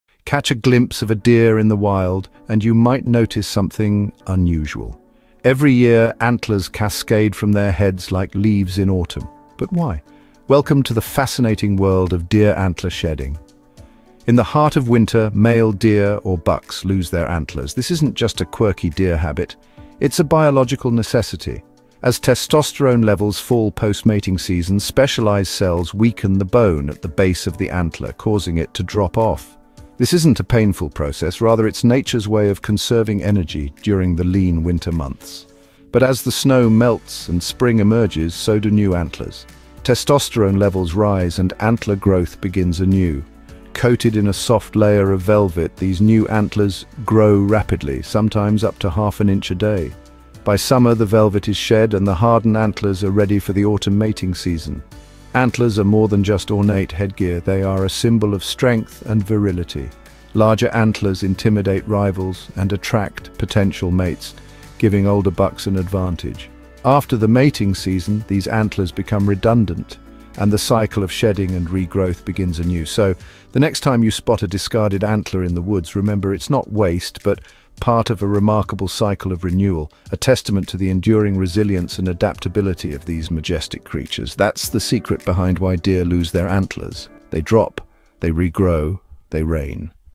Why Do Deer Lose Their Antlers?🦌
Why Do Deer Lose Their Antlers? 🦌 Discover the fascinating reason behind this natural phenomenon! From seasonal changes to survival strategies, find out why deer shed their antlers and what it reveals about their life cycle.